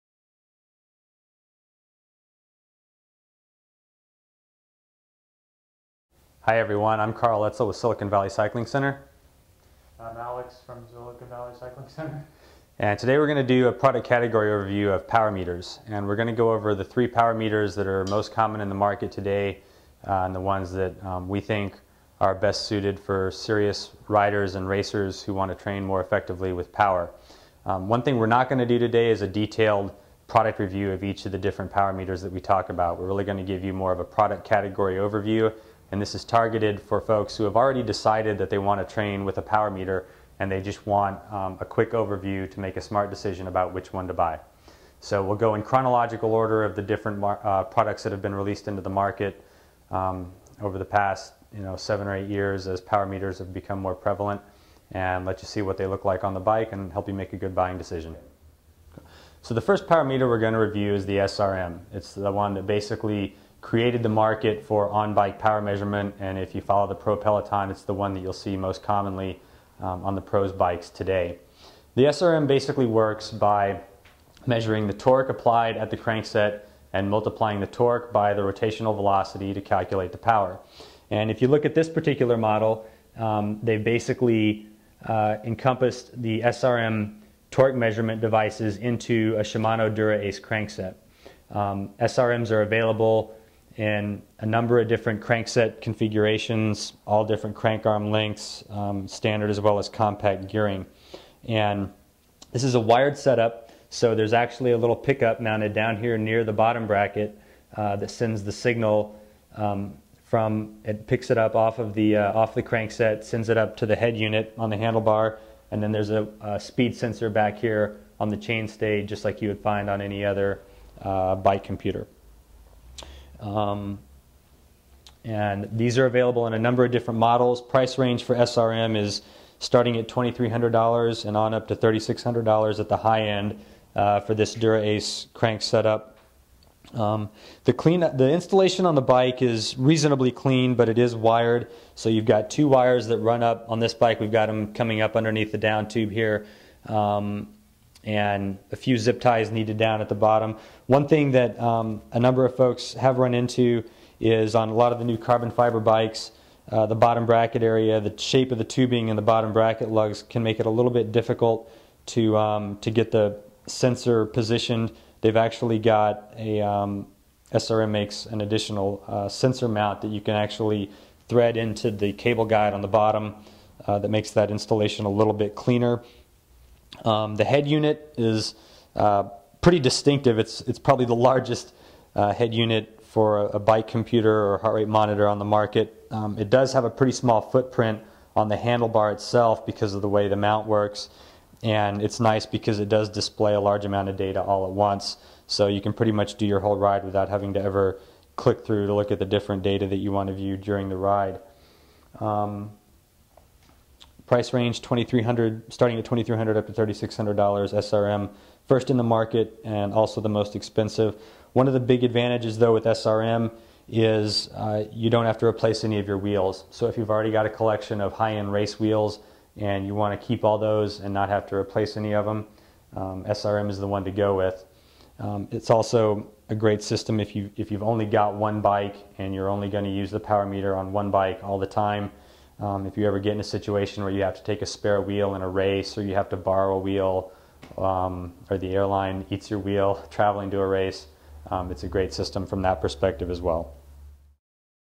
Powermeters - Part 1
Karl and Alex from Silicon Valley Cycling Center introduce 3 of the most popular powermeters available to consumers. Part 1 highlights the SRM Powermeter.